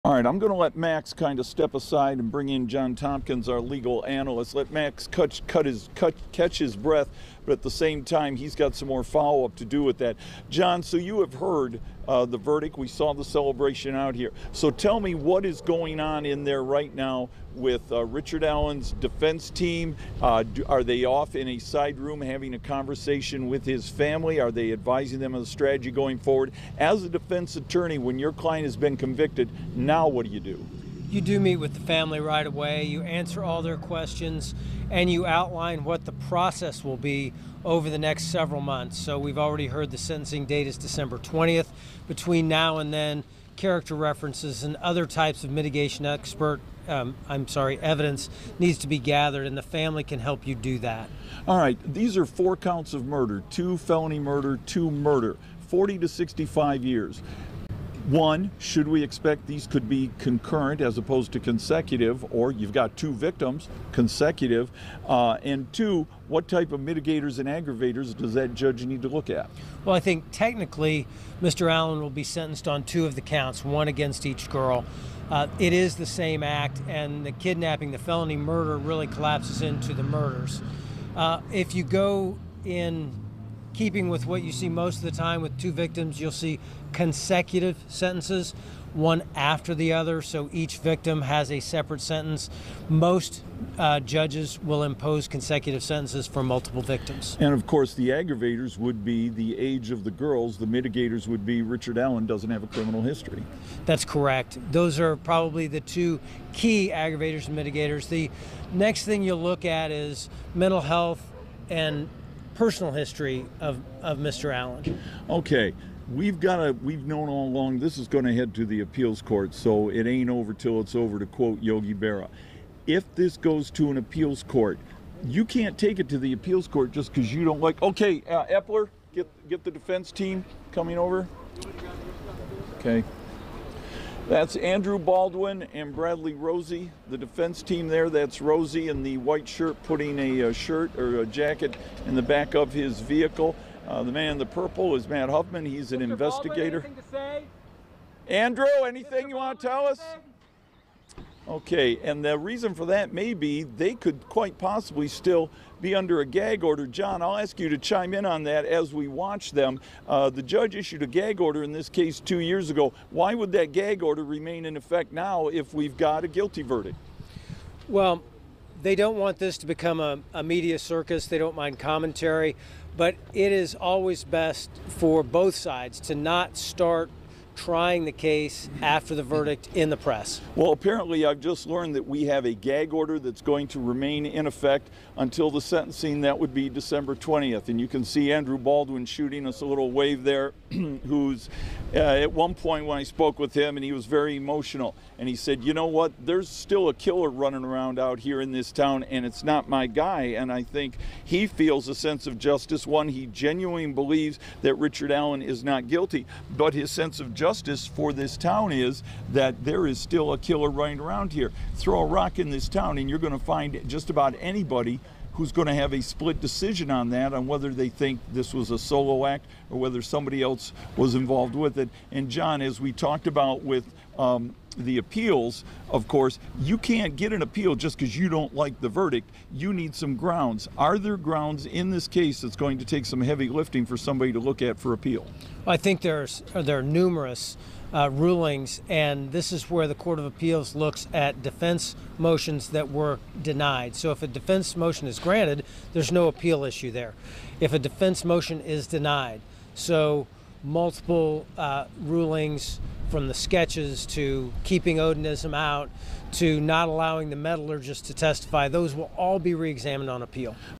Richard Allen found guilty as charged
A jury at the Carroll County Courthouse found Richard Allen, the man accused of killing Abby Williams and Libby German in Delphi in 2017, guilty as charged. Allen will be sentenced on Dec. 20.